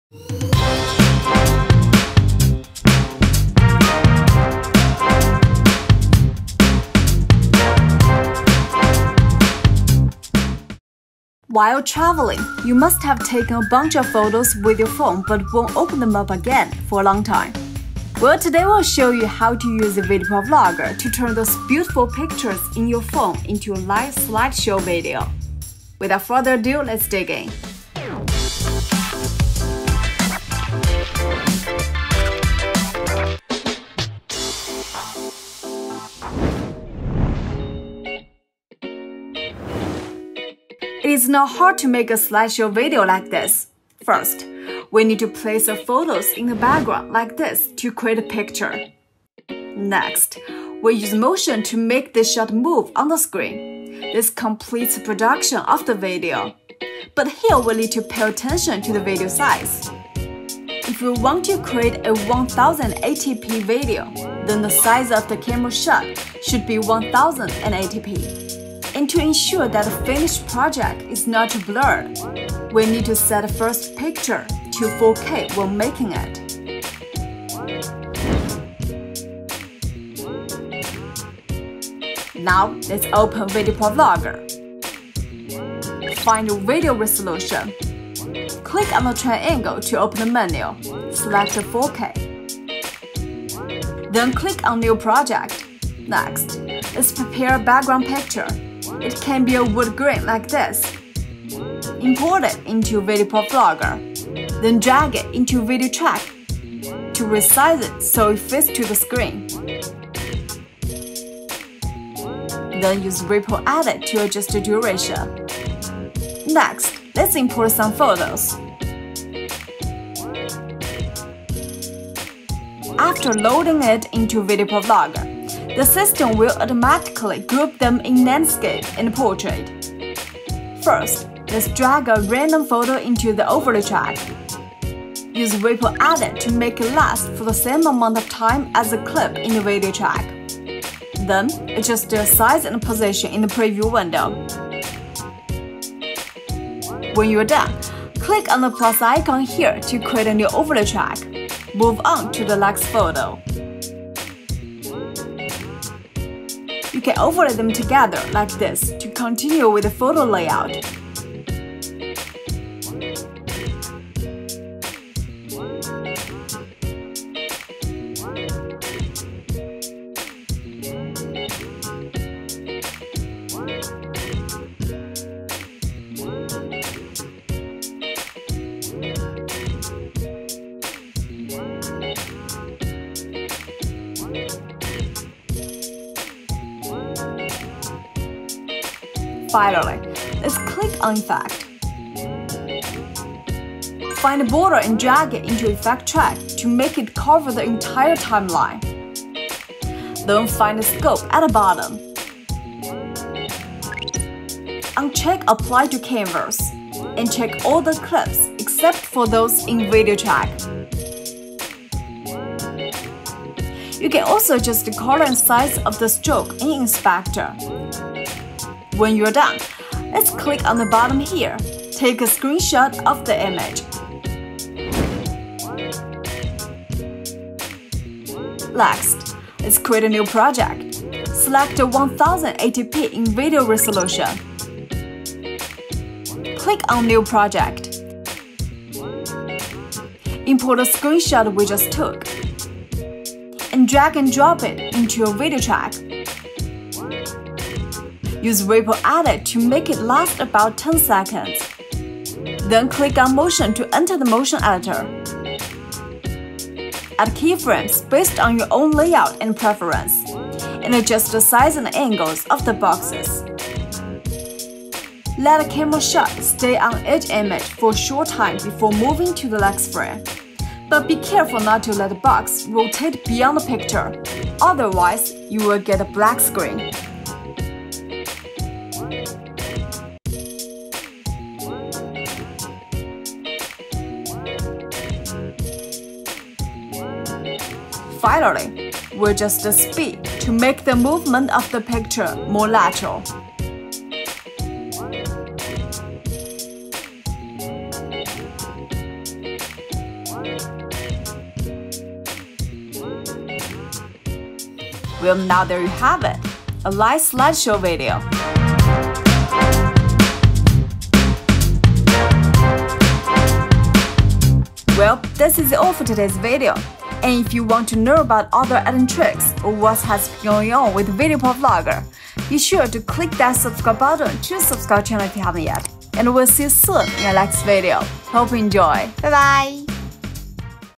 Ep.2 Create a Dynamic BUT Clean Photo Slideshow Video - Best 5 Photo Slideshow Ideas
The Ep.2 of the photo slideshow video is here!!
In the last episode, we have shown how to give your slideshow a simple but impressive look. Well, in today's tutorial, let's try something dynamic, and add to your creation a more dynamic definition but meanwhile keep everything in order. This SOUNDS like a lot of FUN. Then check out the video to learn how to create a dynamic yet well-organized photo slideshow video!

Timestamp:
00:00 Intro
00:39 the principle
01:20 create the picture
03:47 motion